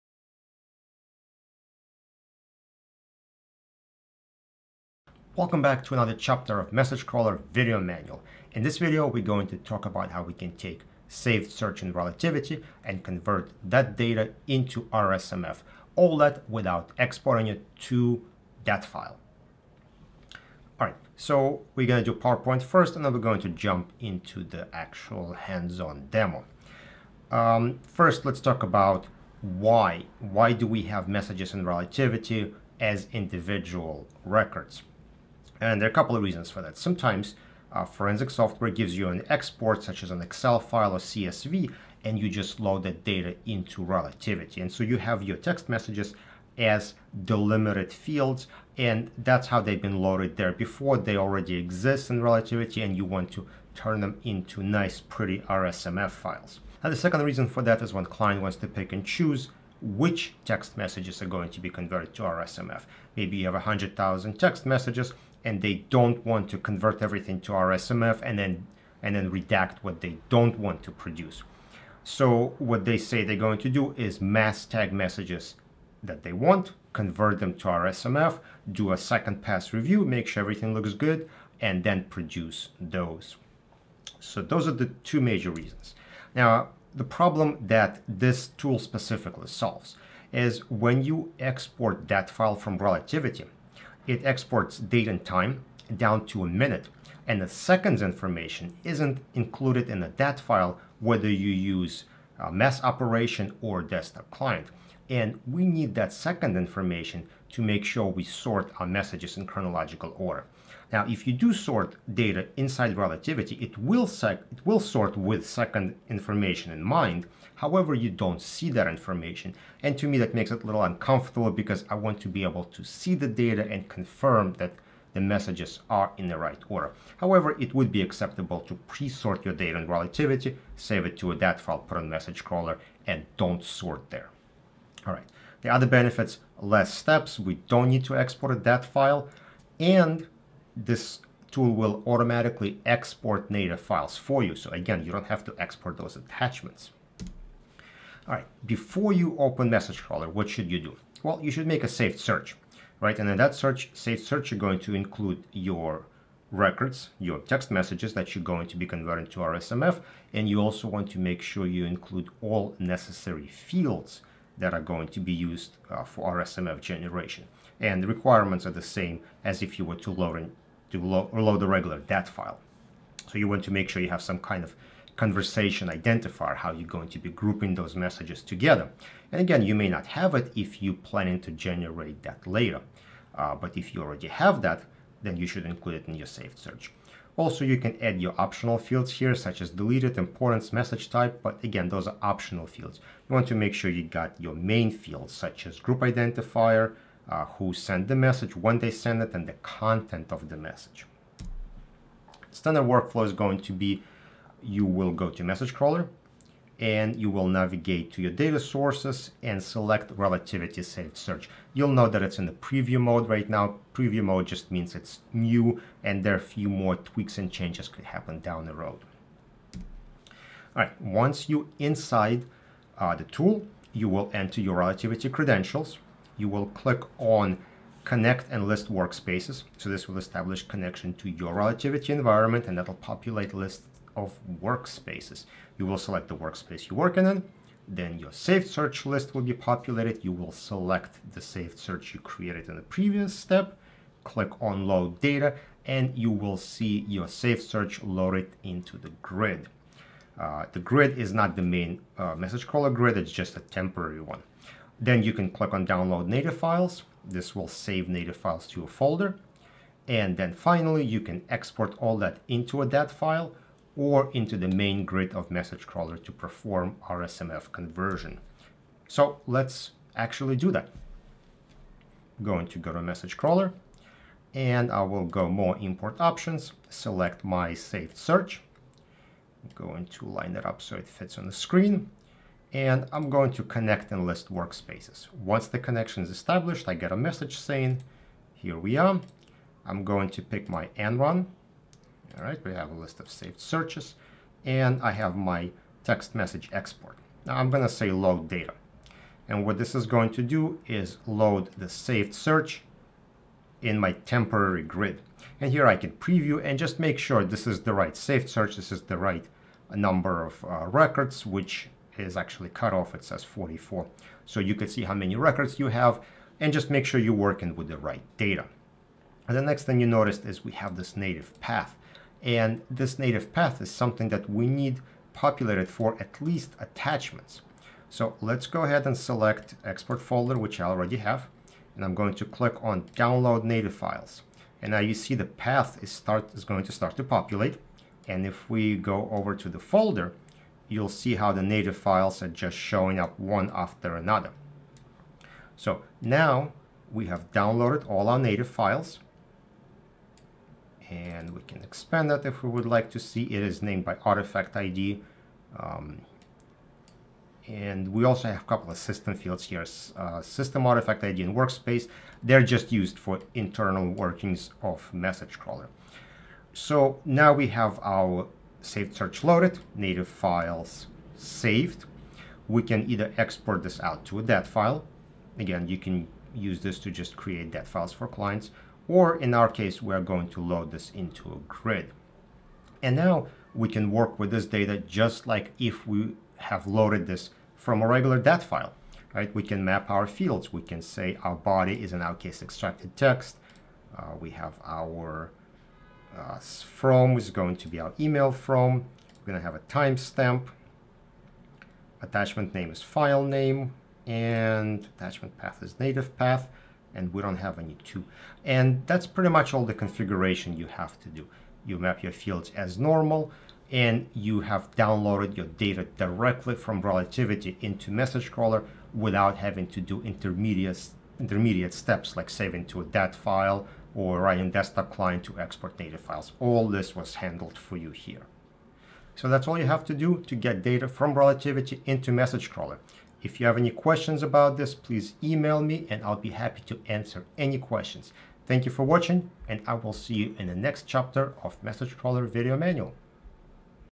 Message Crawler Tools Saved Search to RSMF // Message Crawler Video Manual
What if your text message data was in Excel format and is already loaded to Relativity? Now you need to create RSFM files from that data for easier review or production. Message Crawler can access saved search directly and load data into grid for easy RSMF conversion.